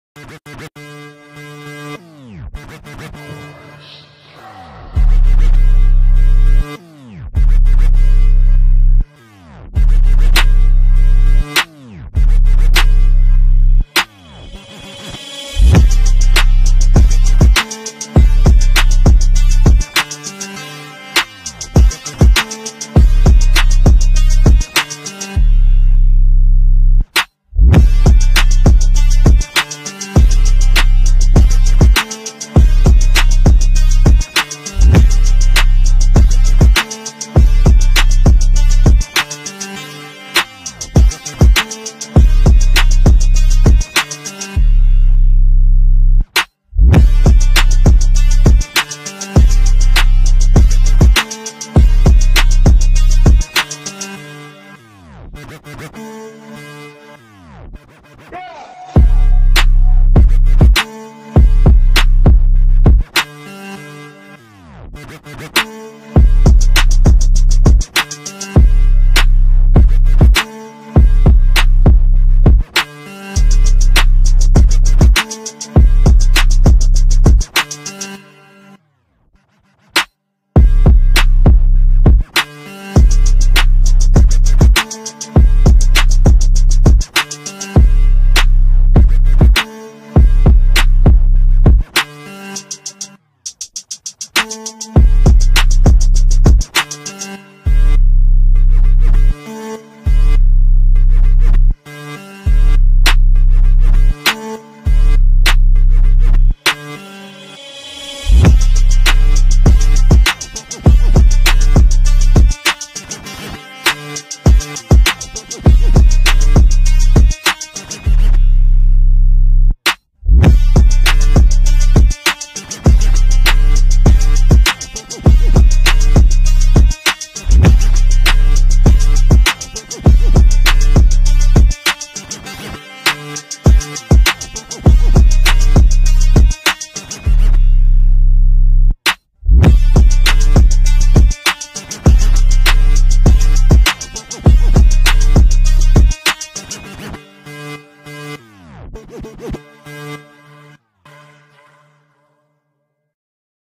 [FREE] SCARLXRD TYPE BEAT 2020 METAL TRAP, DARK UNTAGGED FREE INSTRUMENTAL
This beat in one word: melodies! This trap instrumental has fantastic chords and a great melodic texture to it, which really give it a great foundation for vocals and, in general, melodic artists. That's why I feel Travis Scott and Drake would both murk this beat with their melodic vocals and ad libs. Furthermore, the drop allows for some real bars, as it's carried by the bouncy hi hats and rumbling 808. Enjoy!